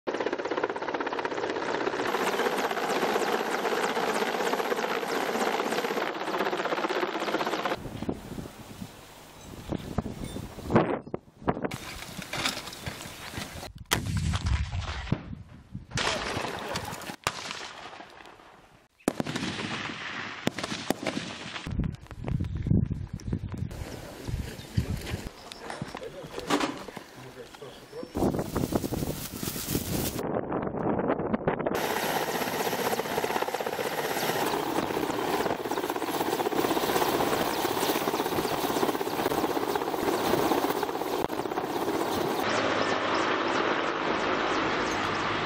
Тактико-специальное учение с подразделениями спецназа ЦВО в Самарской области
Более 500 бойцов спецназа ЦВО разгромили лагерь и стоянку бронетехники условного бандформирования в ходе тактико-специального учения с привлечением армейской авиации в Самарской области

Высадившись беспарашютным способом из вертолетов Ми-8АМТШ в тыл «противника», военнослужащие устроили засаду и уничтожили его бронетехнику, стоявшую в охранении, и разрозненные группировки из РПГ-26 «Аглень» и другого штатного вооружения, захватив при этом важные документы.

Помимо этого, разведчики отработали задачи по эвакуации условно раненых на вертолетах.